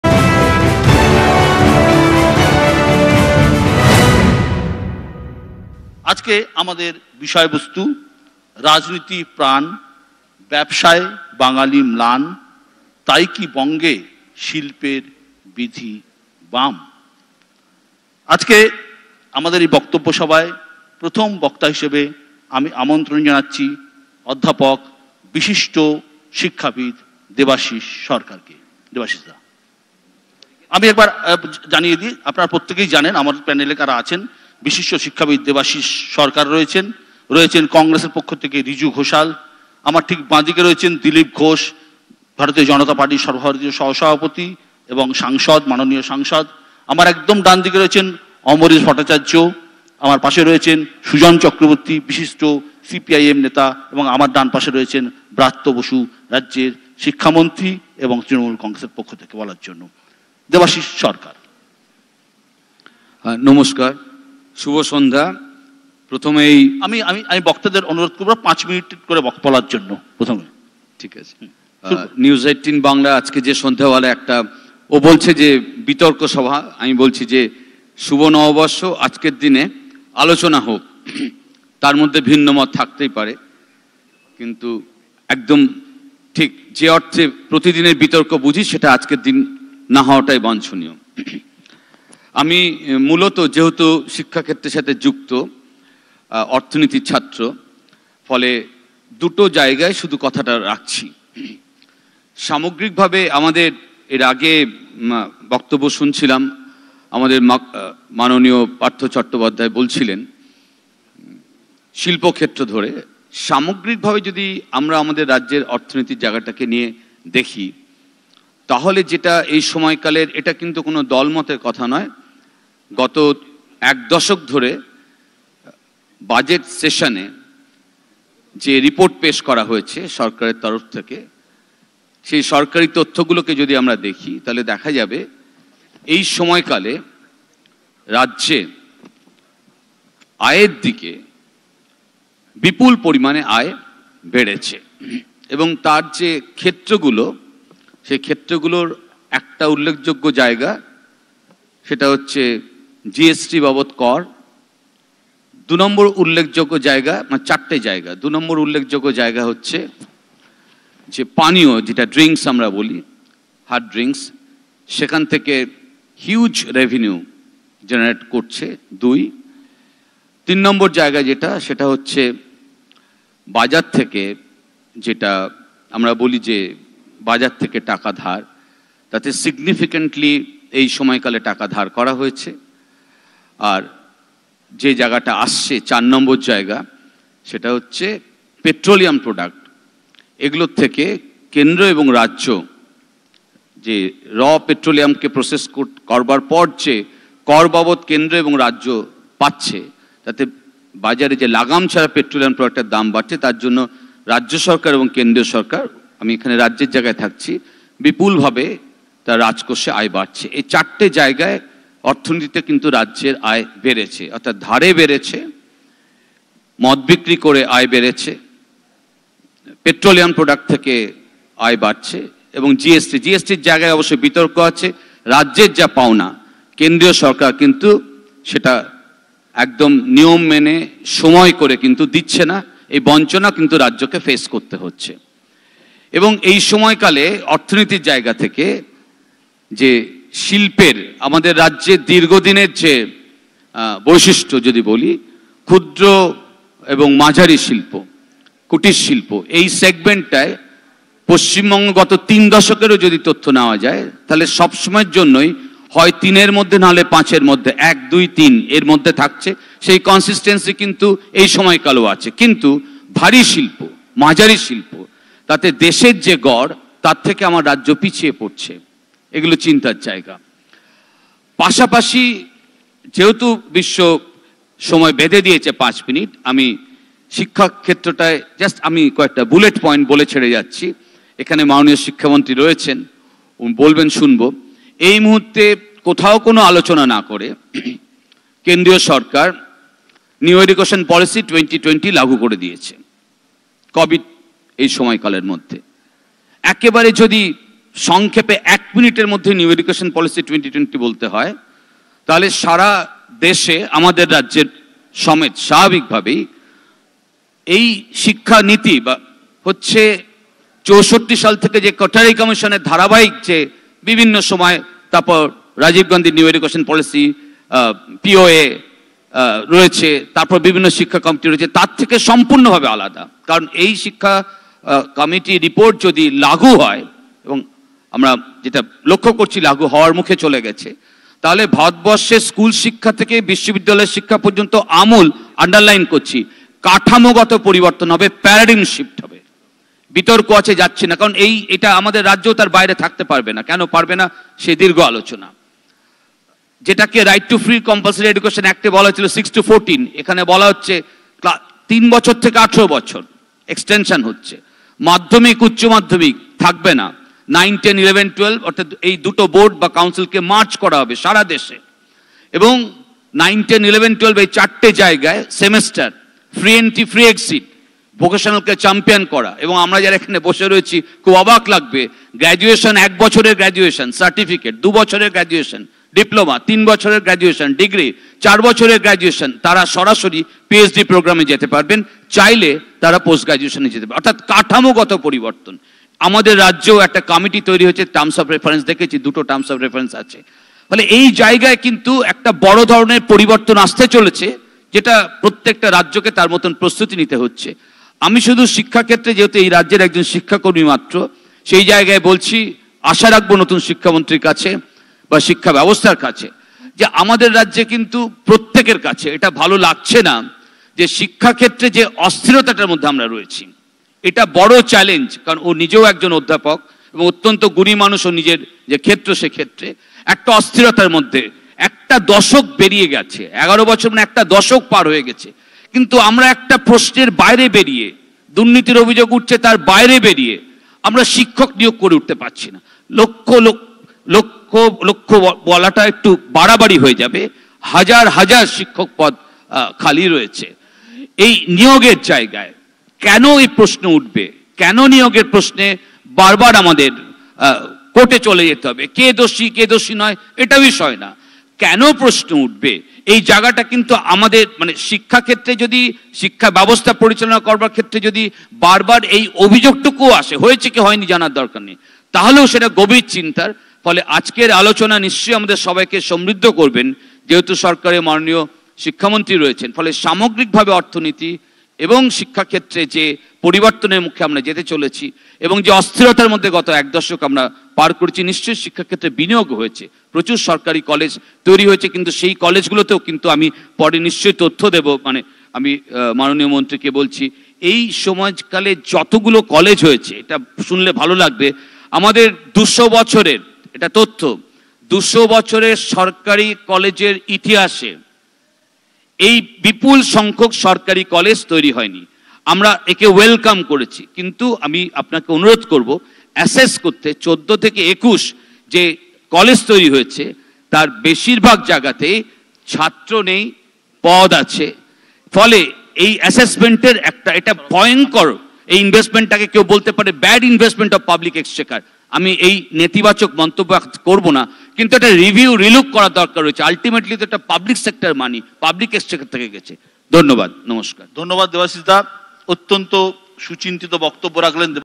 Bengal Conclave 2022: ব্যবসায় বাঙালির ইচ্ছে কতটা, শিক্ষাবিদ Debasish Sarkar কী মনে করেন
Bengal Conclave 2022: Bengal Conclave 2022: ব্যবসায় বাঙালির ইচ্ছে কতটা, শিক্ষাবিদ Debasish Sarkar কী মনে করেন। কী বলছেন Debasish Sarkar?